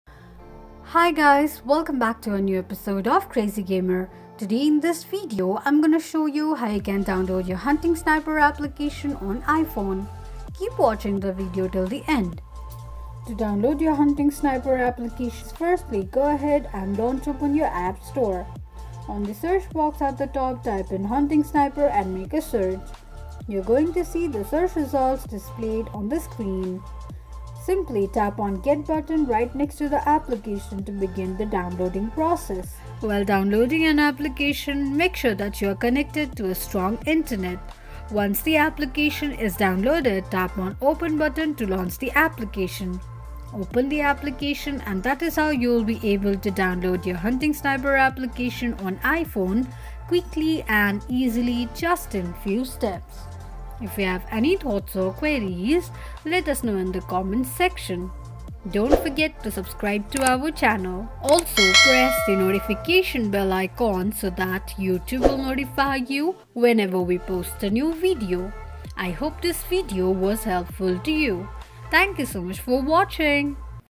How to Download Your Hunting Sniper App on iPhone? Install the Hunting Sniper App 2024
Welcome, fellow hunters! Ready to take your hunting game to the next level? Look no further! In this comprehensive tutorial, we'll walk you through the process of downloading and installing your very own hunting sniper app on your iPhone. Whether you're a seasoned hunter or just starting out, having the right tools at your fingertips can make all the difference.